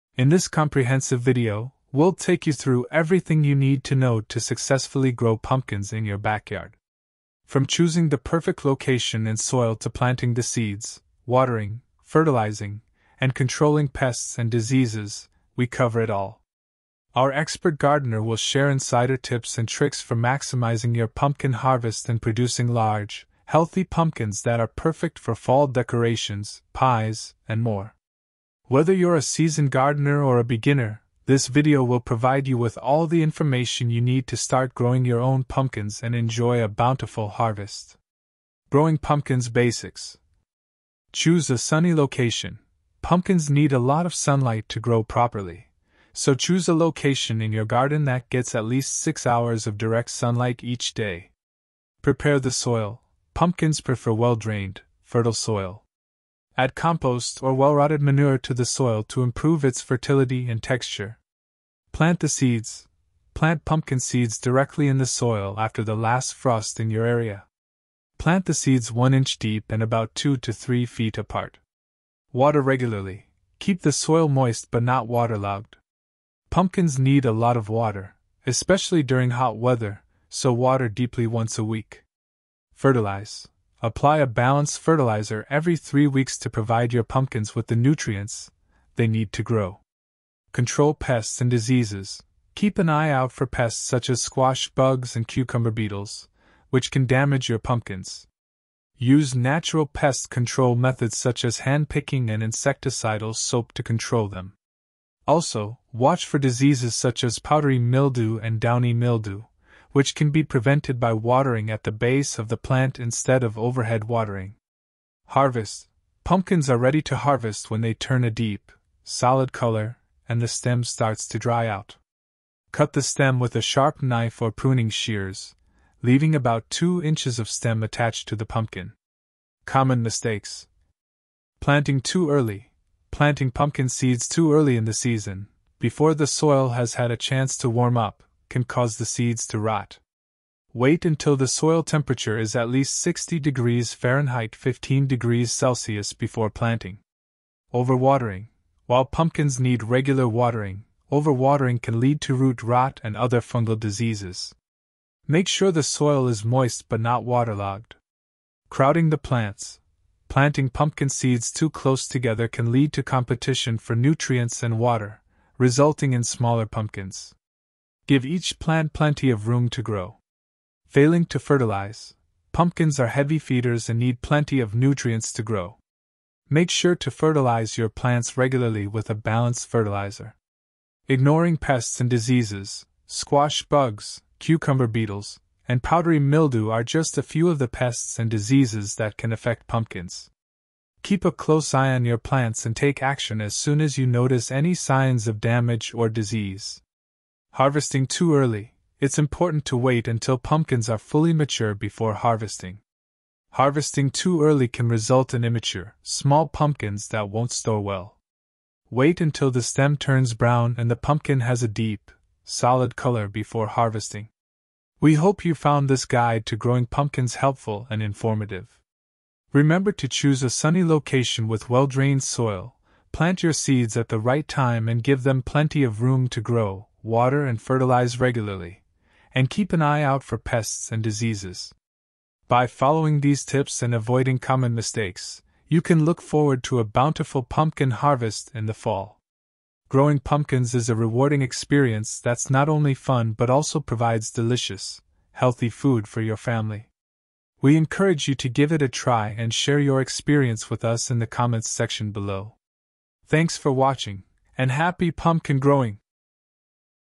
From Seed to Jack-O-Lantern: A Beginner's Guide to Growing Pumpkins
Are you a gardening enthusiast who's eager to try your hand at growing pumpkins? Look no further! In this beginner's guide, we'll take you on a journey from seed to harvest, providing you with all the essential information you need to successfully grow your own pumpkins.

Join us as we delve into the fascinating world of pumpkin cultivation. Learn about selecting the right pumpkin variety, preparing the soil for optimal growth, and the ideal planting techniques to kickstart your pumpkin patch. We'll also share expert tips on watering, fertilizing, and maintaining your plants throughout the growing season.

Discover the common challenges faced by beginners and gain valuable insights on pest and disease management, ensuring your pumpkins stay healthy and vibrant. You'll also find guidance on when and how to harvest your pumpkins, ensuring they reach their full potential for fall decorations, delicious pies, and seasonal festivities.

Whether you have a small backyard garden or ample space in a community plot, this beginner's guide will equip you with the knowledge and confidence to grow pumpkins successfully. Join us on this horticultural adventure and unlock the joys of cultivating your very own pumpkins!